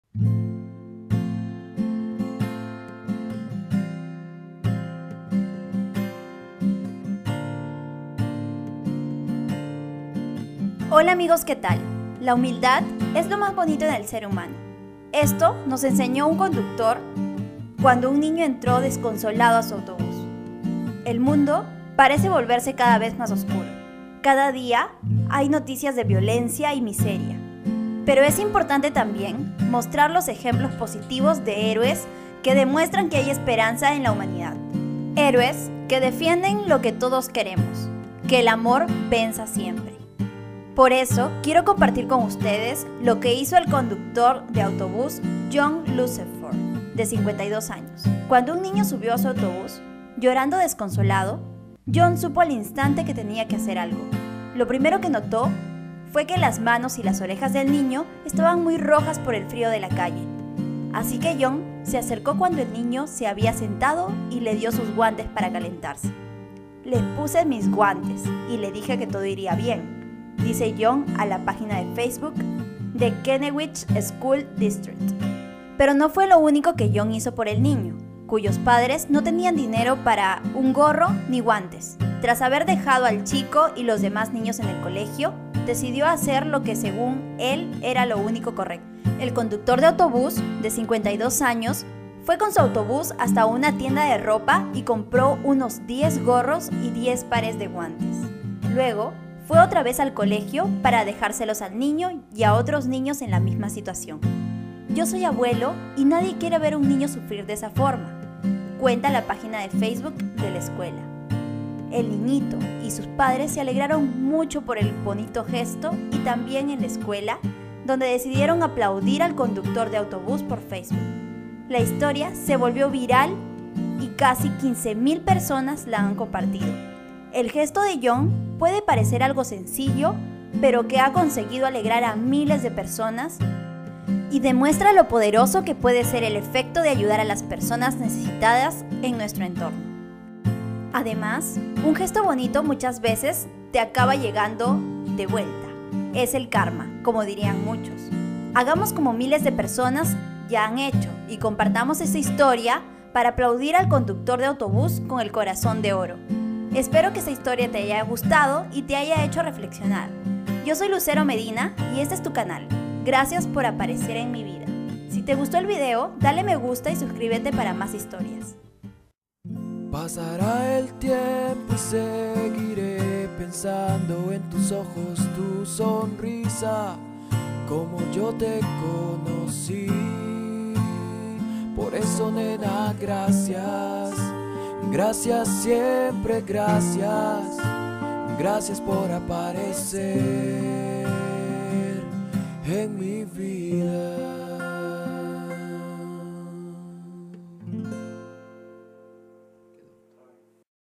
El Chico Se Sube Llorando Al Autobús Cuando El Conductor Descubre El Motivo Actúa Inmediatamente.
Hola amigos que tal? La humildad es lo más bonito en el ser humano, esto nos enseñó un conductor de autobús con un niño que entró desconsolado a su autobús.
El mundo parece volverse cada vez más oscuro, cada día hay noticias de violencia y miseria, pero es importante también mostrar los ejemplos positivos de héroes que demuestran que hay esperanza en la humanidad. Héroes que defienden lo que todos queremos “que el amor venza siempre”.
Por eso quiero compartir con vosotros lo que hizo el conductor de autobús John Lunceford, de 52 años.
Cuando un niño subió a su autobús, llorando desconsolado, John supo al instante que tenía que hacer algo. Lo primero que notó fue que las manos y las orejas del niño estaban muy rojas por el frío de la calle, así que John se acercó cuando el niño se había sentado y le dio sus guantes para calentarse.
“Le puse mis guantes y le dije que todo iría bien”, dice John a la página de Facebook de Kennewick School Districts.

Pero no fue lo único que John hizo por el niño, cuyos padres no tenían dinero ni para gorro ni guantes, tras haber dejado al chico y los demás niños en el colegio, decidió hacer lo que según él era lo único correcto.
El conductor de autobús de 52 años fue con su autobús hasta una tienda de ropa y compró unos diez gorros y diez pares de guantes, luego fue otra vez al colegio para dejárselos al niño, y a otros niños en la misma situación.
“Yo soy abuelo y nadie quiere ver a un niño sufrir de esa forma “, cuenta la página de Facebook de la escuela.
El niñito y sus padres se alegraron mucho por el bonito gesto, y también en la escuela, donde decidieron aplaudir al conductor de autobús por Facebook. La historia se volvió viral y casi 15 000 personas la han compartido.
El gesto de John puede parecer algo sencillo pero que ha conseguido alegrar a miles de personas, y demuestra lo poderoso que puede ser el efecto de ayudar a las personas necesitadas en nuestro entorno. Además un gesto bonito muchas veces te acaba llegado del vuelta, es el karma, como dirían muchos.
¡Hagamos como miles de personas ya han hecho y compartamos esta historia para aplaudir a este conductor de autobús con el corazón de oro!